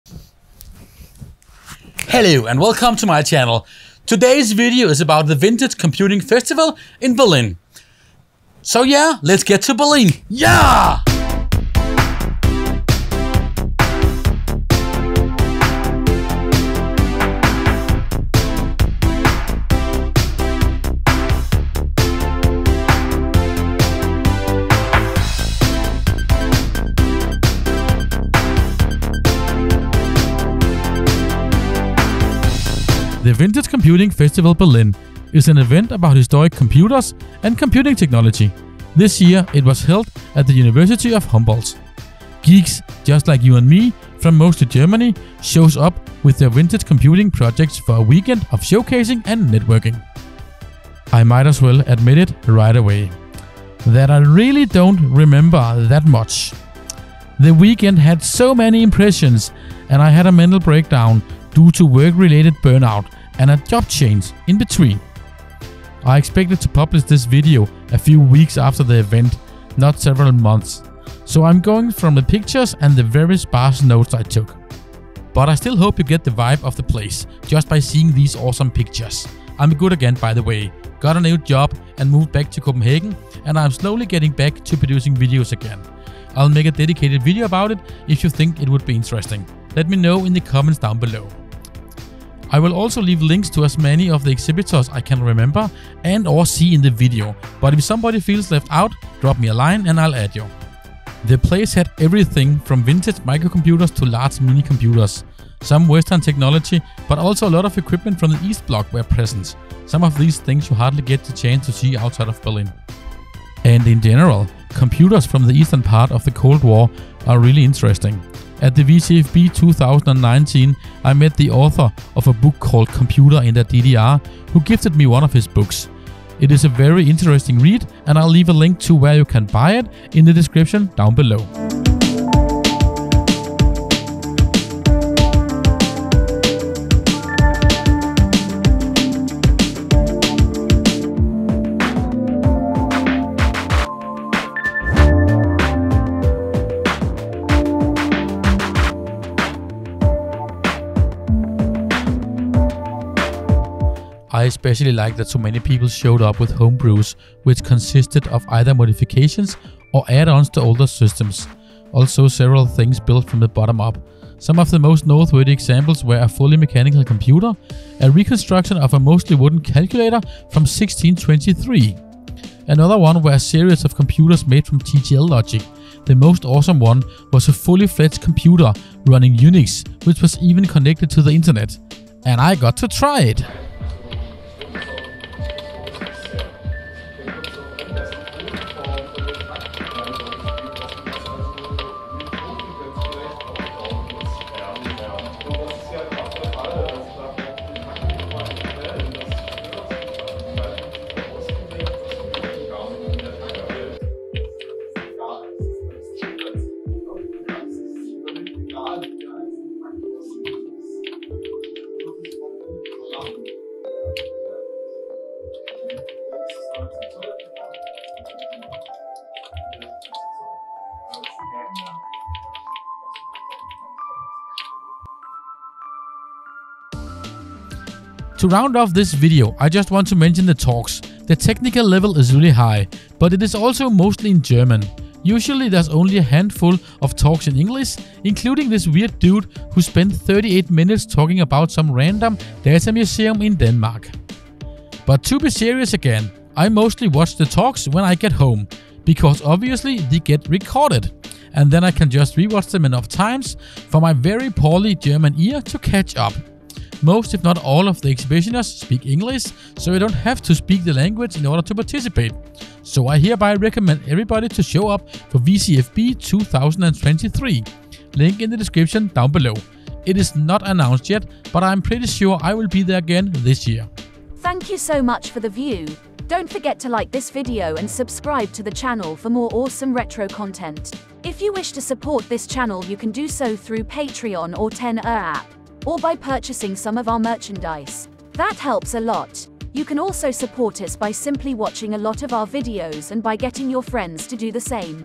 Vintage Computing Festival Berlin 2022
The Vintage Computing Festival Berlin, is an event about historic computers and computing technology, this year it was held at the University of Humboldt.

Geeks just like you and me from mostly Germany shows up with their vintage computing project for a weekend of showcasing and networking.

0:00 Intro
0:32 The exhibiton
1:58 Computer in der DDR
2:44 Wooden computerRegaining control of the game publishing for their platform
2:52 TTL Based UNIX System
3:48 The Talks

Links: (In order of mention)

Computer in der DDR